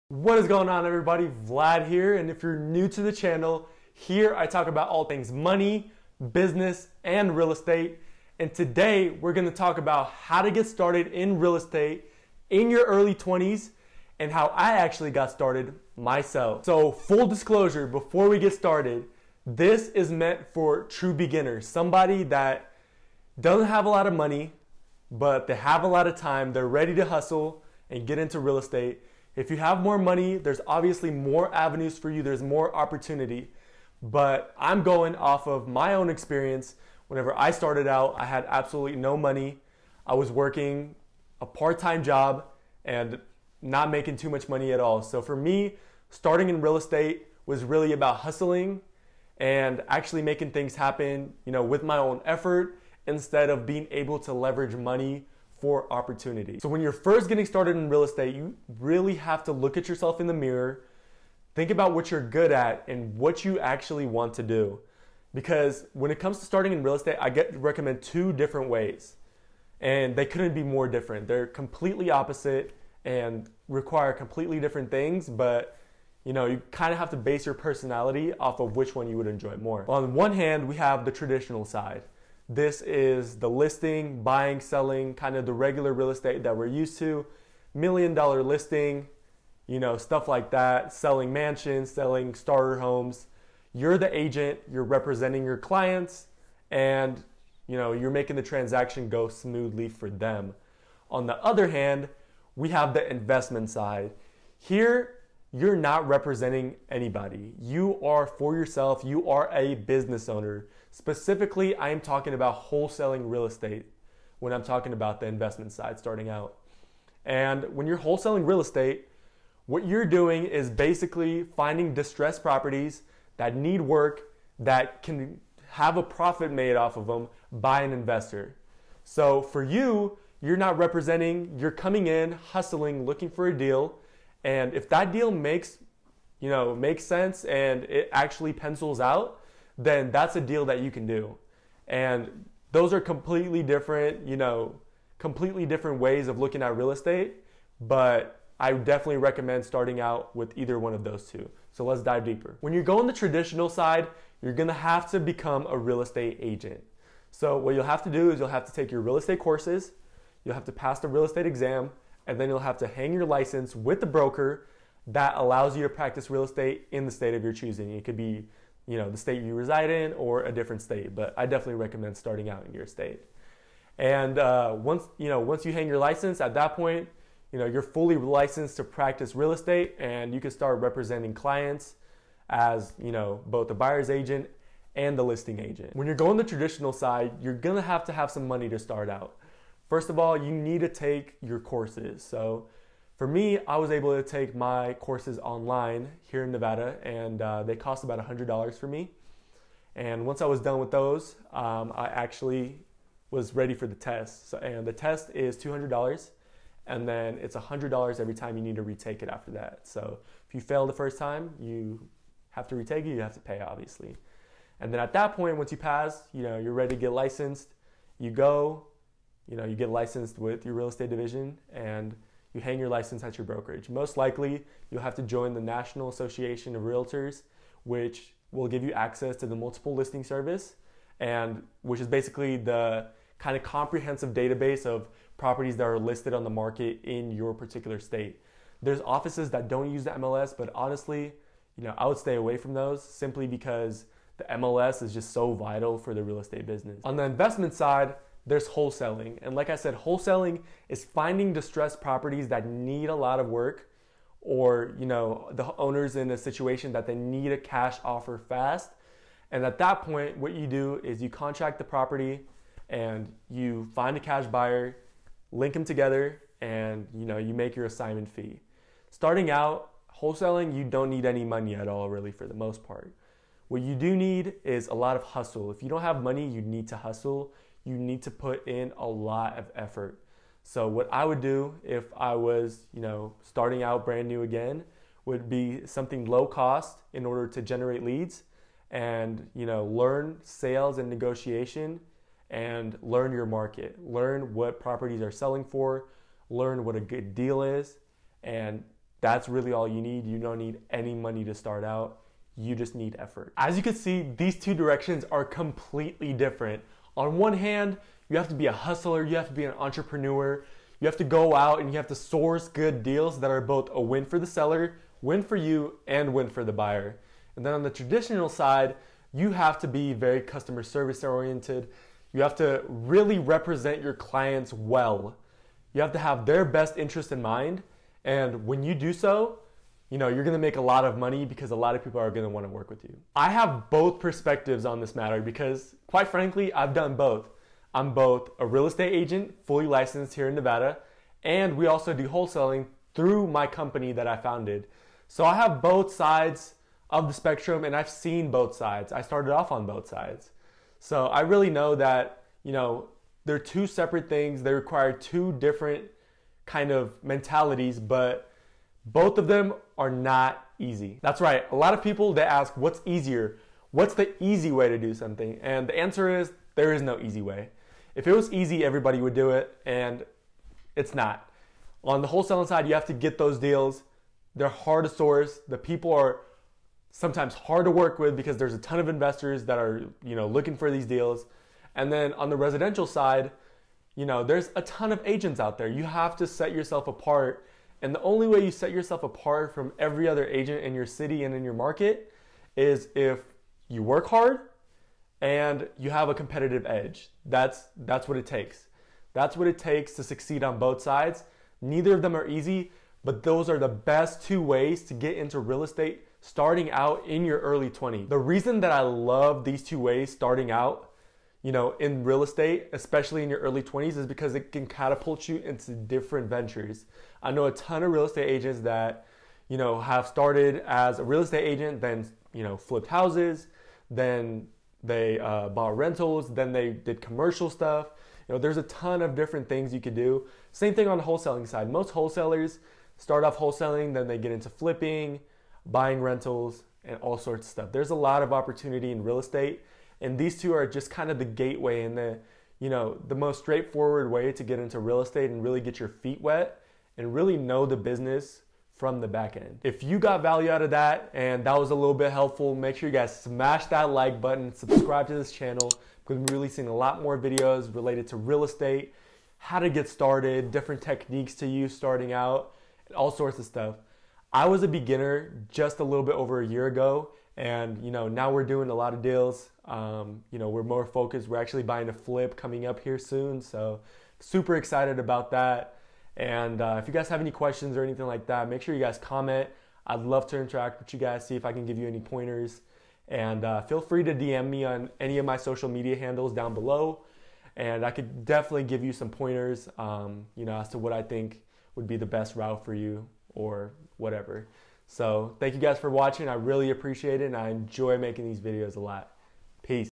The Easiest Ways To Get Started In Real Estate
What are the easiest ways to get started in real estate? Ever since I've set out in real estate many people have reached out to me asking to help them get started in real estate. It is an attractive industry and many people want to start and get deterred. After all, there is a stigma that you have to be older and wealthy to get started. But my goal is to really dispel that myth and show you guys that you don't need much money to get started and be successful in the industry.

If you have the drive, savvy and people skills you will not only succeed but thrive in the industry and build up your wealth. I think it is a viable option for anyone looking to make a positive change in their life, I know I have.

Now I don't recruit sell, sell a course or any of that, frankly it doesn't matter if you choose to pursue real estate. All I can say is how real estate has impacted me and how I am going to build my career moving forward.

Once you learn the basics of real estate in your 20's you will be well equipped to tackle any investment, sale or transaction in your life. It really builds on itself as you move forward and that is the beauty of real estate.

I look forward to interacting with everyone and seeing how I can help out.